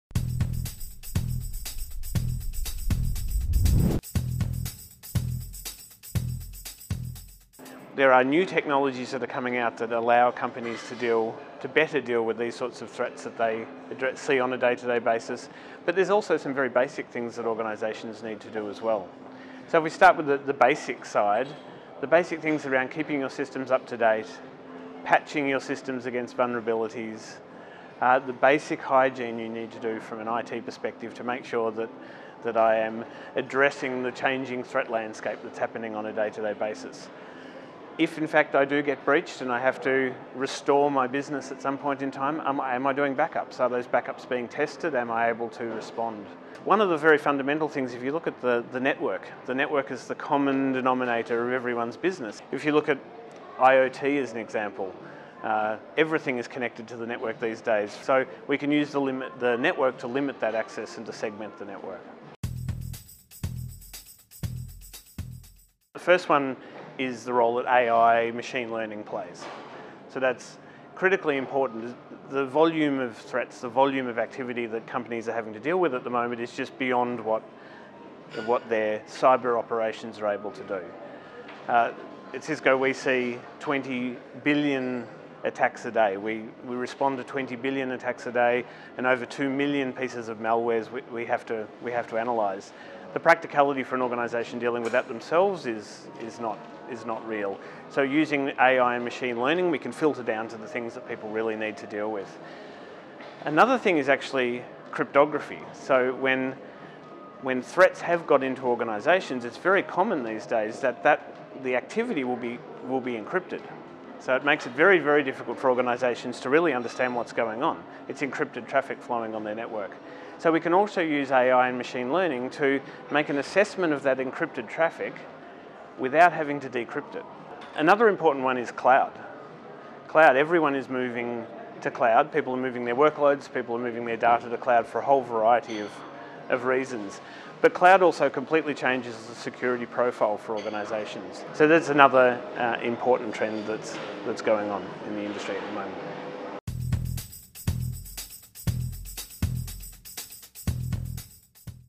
New Tech New Threats : Top 3 Tech Trends In Cybersecurity Today | Edge Bites
Learn the latest cybersecurity trends and best practises in order to keep pace with the ever-evolving technology. Find out more as we speak with Andrew Hobby from Cisco.